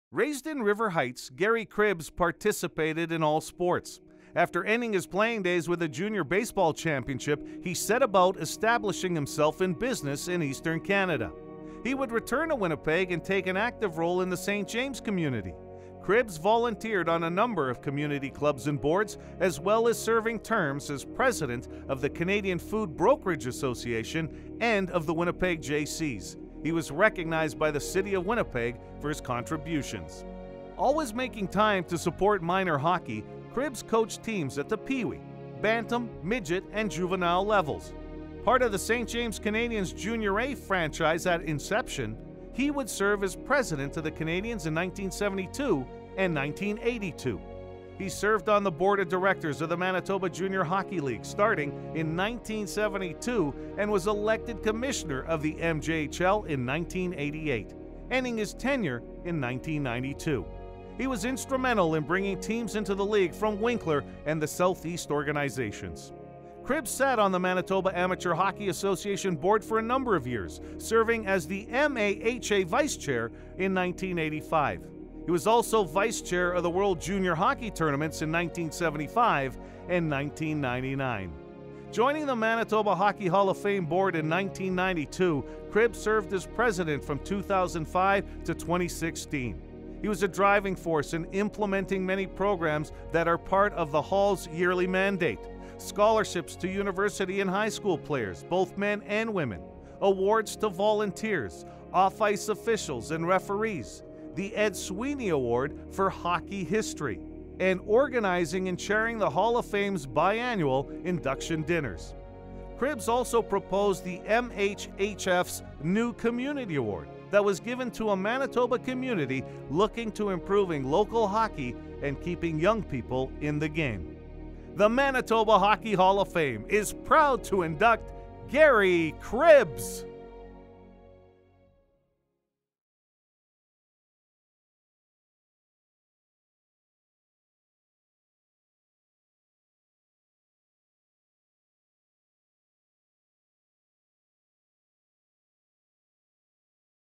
Gary Cribbs
Raised in River Heights, Gary Cribbs participated in all sports. After ending his playing days with a Junior baseball championship he set about establishing himself in business in Eastern Canada.

He would return to Winnipeg and take an active role in the St. James community. Cribbs volunteered on a number of community clubs and boards, as well as serving terms as President of the Canadian Food Brokerage Association and of the Winnipeg Jaycees. He was recognized by the City of Winnipeg for his contributions.

Always making time to support minor hockey, Cribbs coached teams at the Pee Wee, Bantam, Midget and Juvenile levels. Part of the St. James Canadians Junior A franchise at inception, he would serve as President of the Canadians in 1972 and 1982.
He served on the Board of Directors of the Manitoba Junior Hockey League, starting in 1972 and was elected Commissioner of the MJHL in 1988, ending his tenure in 1992. He was instrumental in bringing teams into the League from Winkler and the South East organizations.